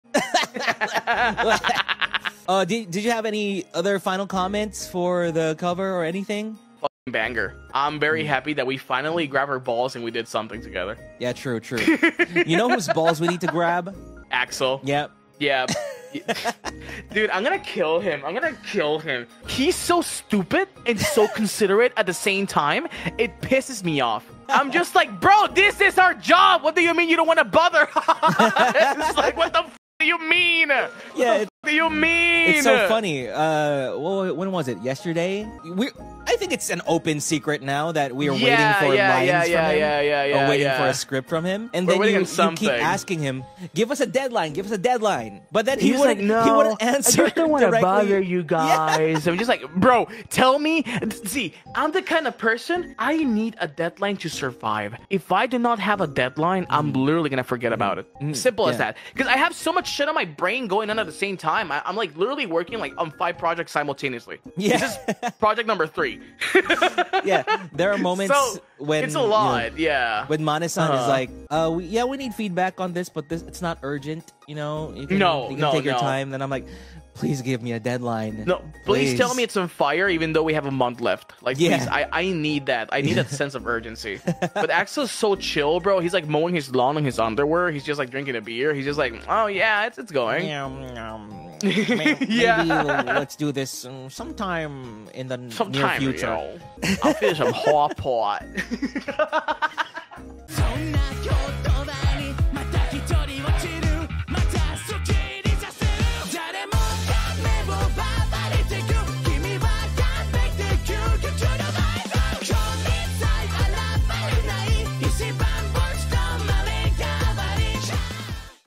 Octavio and Hakka will grab Axel by the BALLS until they make music! 【Holostars EN
Damn guy, chill 💀